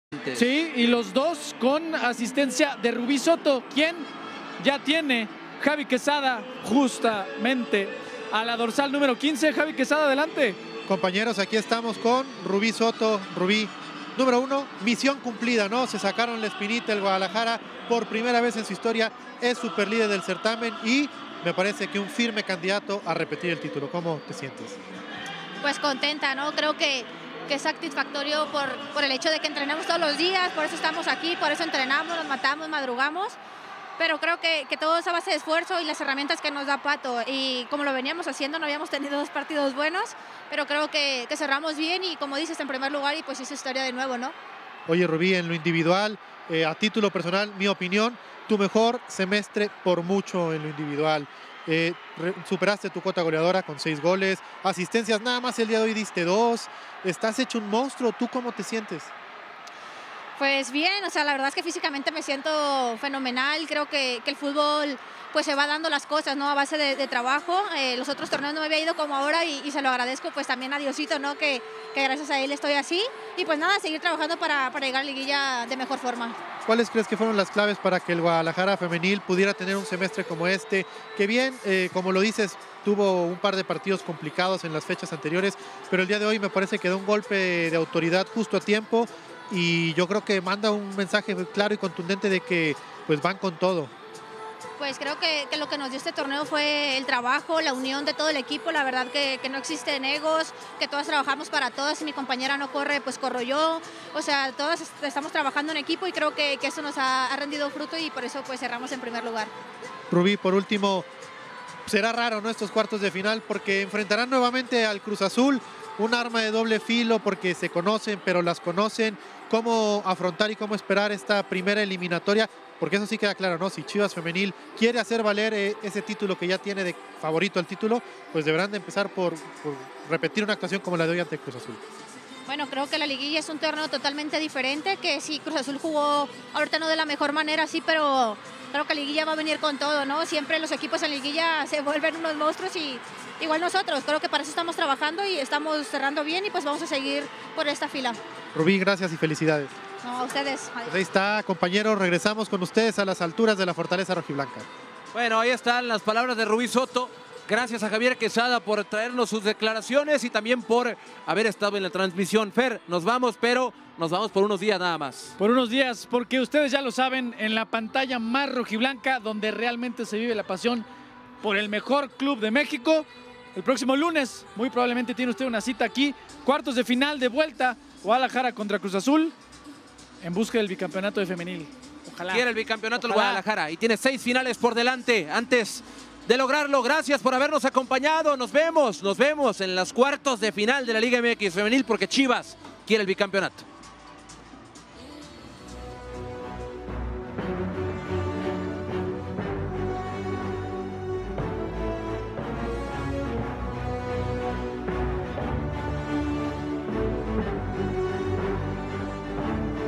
Rubí Soto, ¿Qué dijo tras el triunfo de Chivas Femenil sobre Cruz Azul? 👀
🚨 Conviértete en miembro del canal más Rojiblanco para disfrutar contenido exclusivo de CHIVASTV ➡️
Rubí Soto, ¿Qué dijo tras el triunfo de Chivas Femenil vs Cruz Azul? 👀